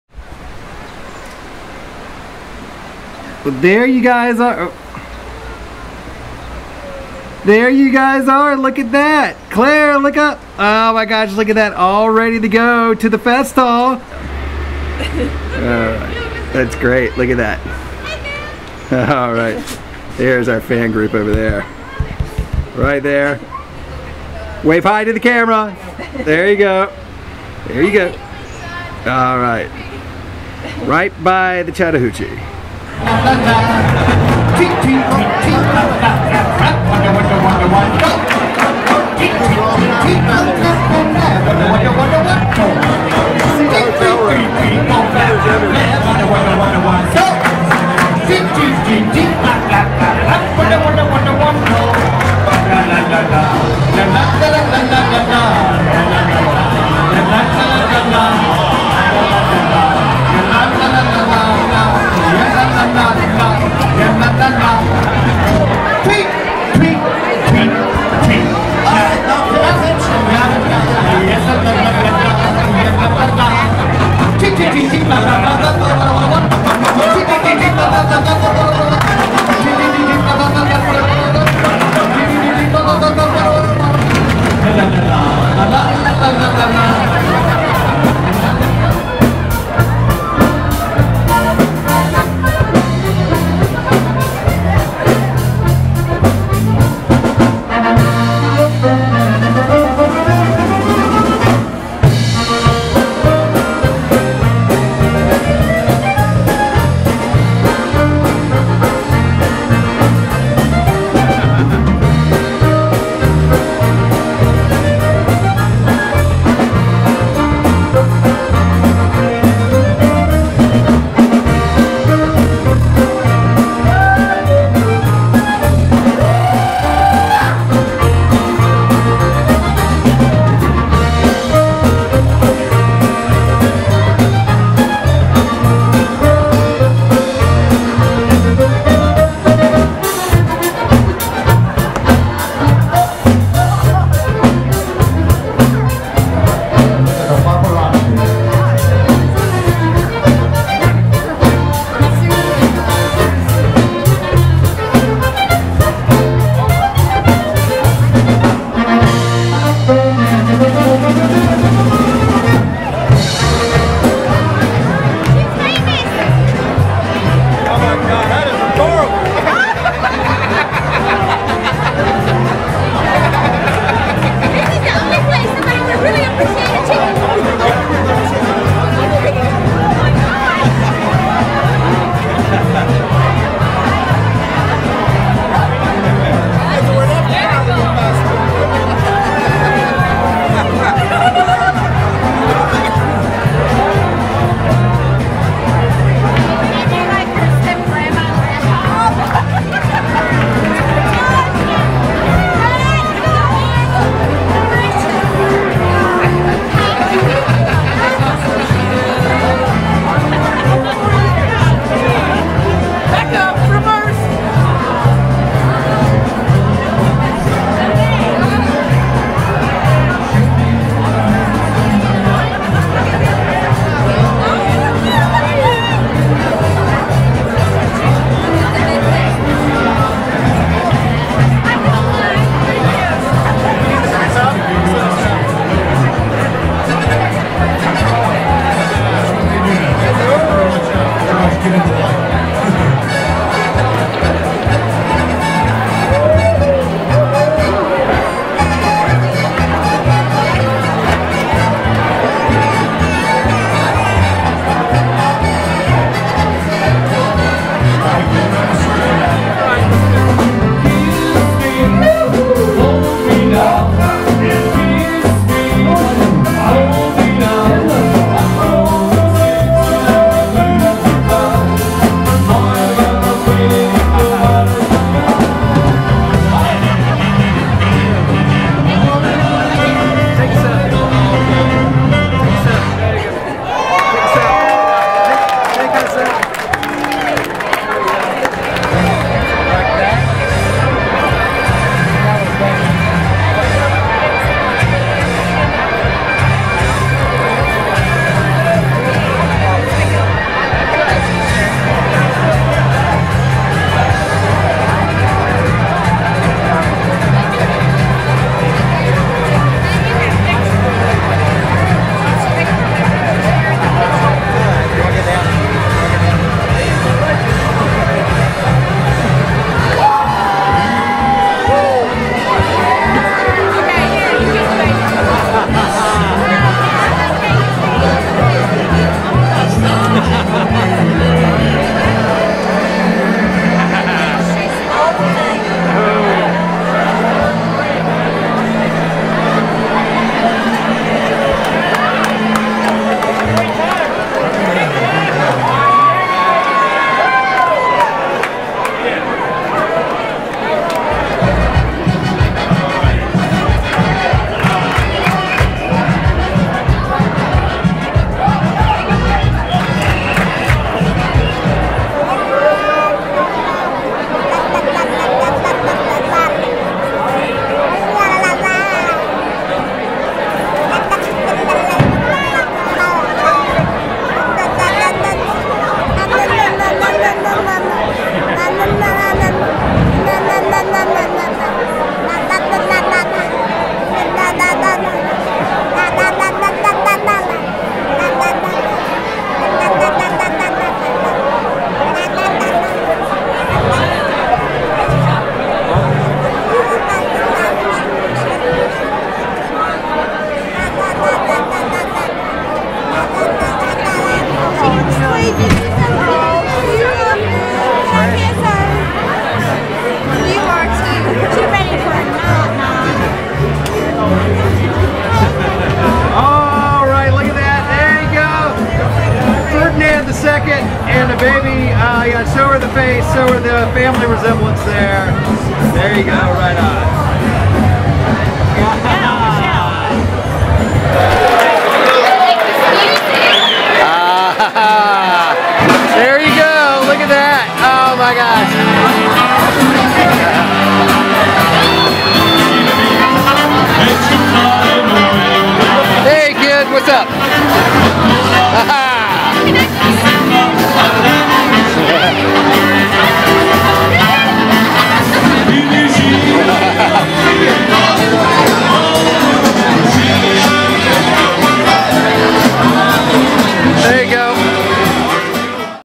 RE-UPPED: Oktoberfest 2012 Festhall Night Two RECOLORED
Already posted this, I wanted to press the human limits of turning up the color saturation (except on skin tones) to mimic sitcom coloring!  ...or whatever.